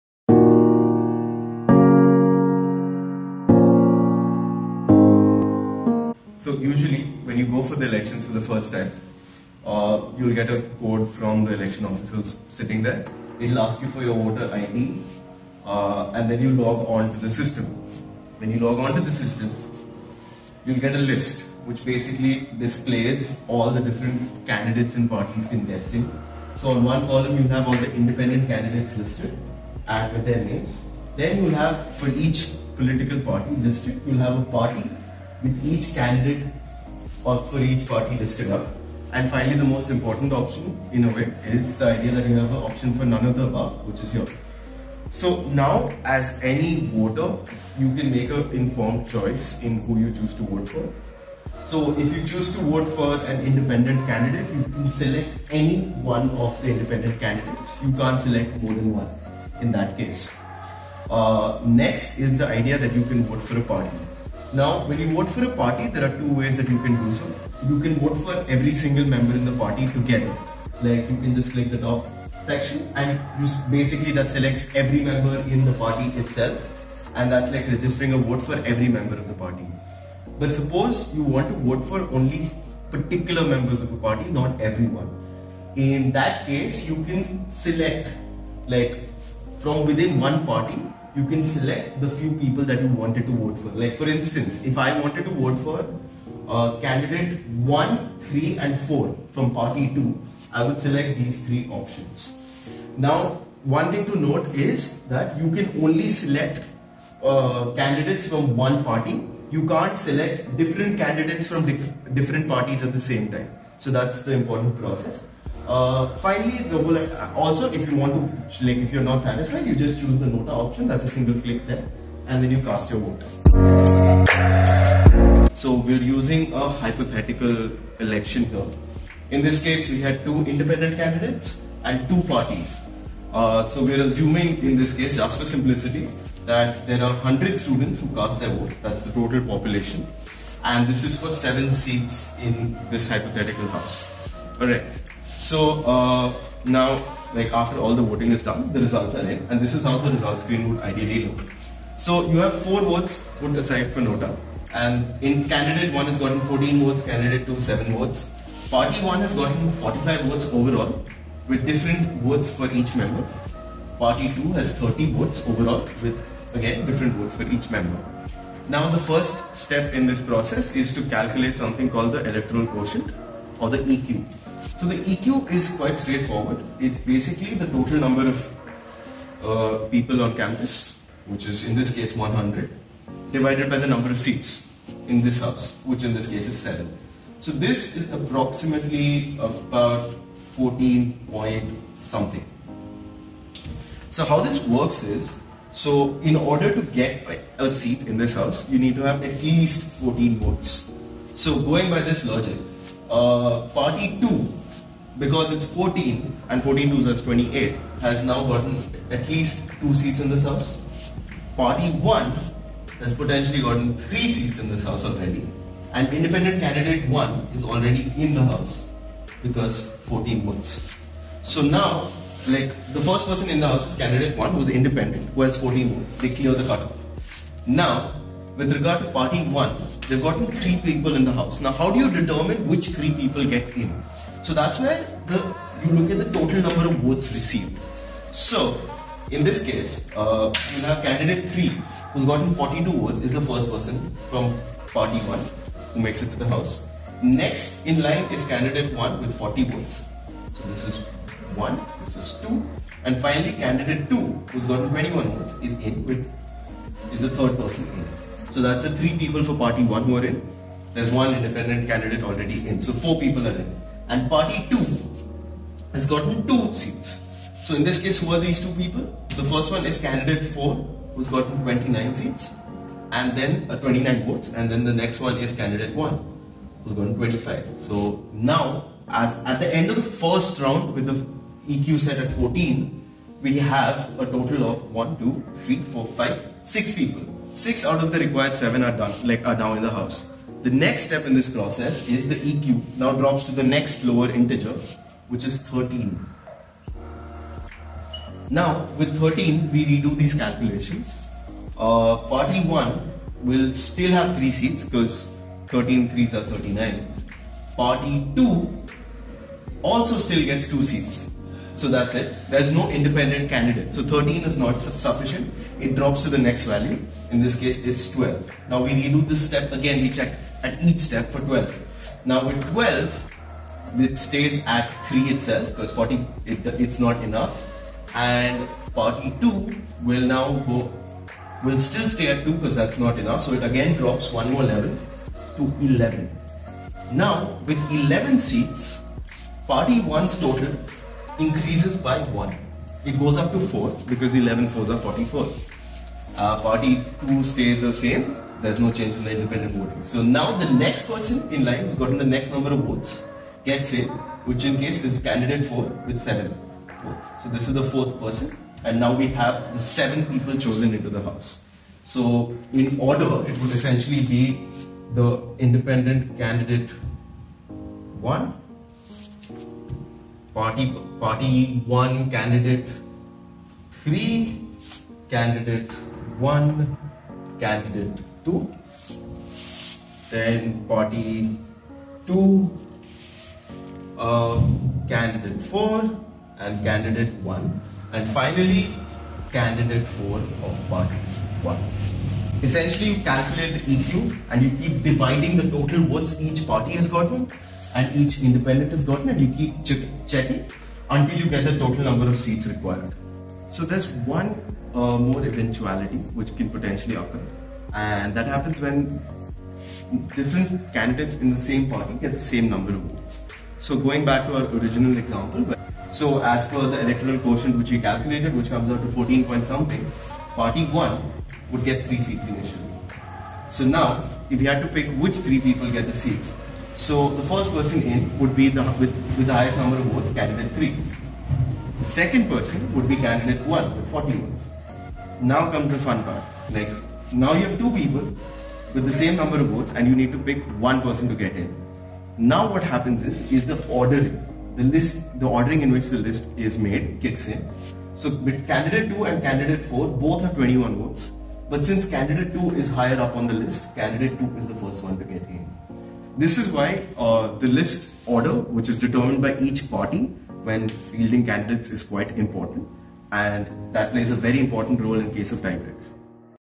How Ashoka Votes
Apuroop Sethupathy explains the voting process at Ashoka University. The system was designed in consultation with the Political Science Department, and the software was designed by Paras Bhattrai.

Credits:
Video: Abhinav Verma
Post-processing: Shreshth Budhiraja